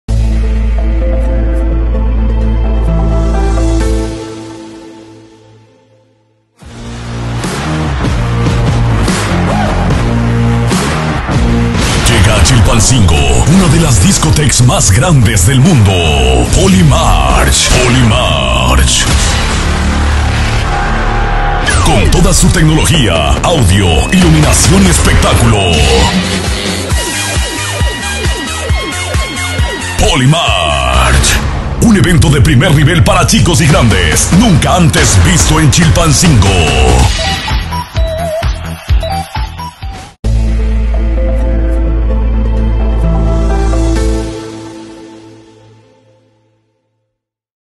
19 de diciembre la presentación de Polymarchs EN CHILPANCINGO
No te puedes perder este 19 de diciembre la presentación de Polymarchs, una de las discotecas más grandes del mundo. La cita es en la cancha de la colonia los Ángeles a partir de las 6 de la tarde.

Adquiere tus boletos a partir de ya entregando un producto de la canasta básica en las oficinas del patronato de la Feria de Chilpancingo, San Mateo, Navidad y Año Nuevo ubicadas en la calle Acapulco S/N Colonia Indeco (como referencia atrás del centro de salud ). Todo lo recaudado será para ayudar a nuestras y nuestros hermanos afectados  por el huracán "Otis"